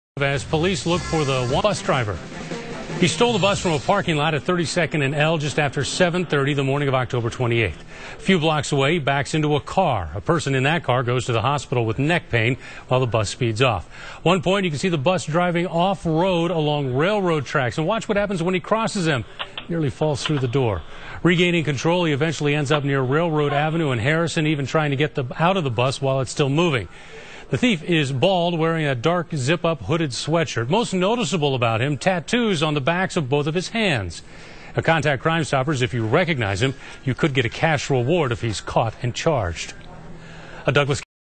Crime of The Week - 11-17-2011
On Friday, October 28, 2011 at approximately 7:40 a.m. a yellow school bus was taken from the parking lot of 3225 L Street, Hardee's, by an unknown black male.  At 8:30 a.m. a hit and run personal injury accident was reported at 25th and Z Street.  The driver told officers that he was behind a yellow school bus, when it back into his car and then pulled forward to get around traffic that wasn't moving.  The vehicles passenger complained of neck pain and was transported to the hospital.  Cameras inside the bus captured the suspect on his joyride.  FrontView and SideView video files best show the suspects face at the 3:33 time frame.  On the SideView camera at 4:42 you can see the suspect has tattoos on the top of both hands.  On the same video you can see the bus goes off-road at 10:46 and at 11:29 the suspect almost falls out.

The bus was abandoned by the suspect in the area of Railroad Avenue and Harrison Street.  In the video, the suspect can be seen attempting to exit the bus while it was rolling away (SideView 13:35).  It appears the bus was eventually located with the help of a GPS tracking device.

The suspect appears to be a Hispanic male or light skinned black male and bald wearing a dark colored zip-up hooded sweatshirt.

RB# 42512-Z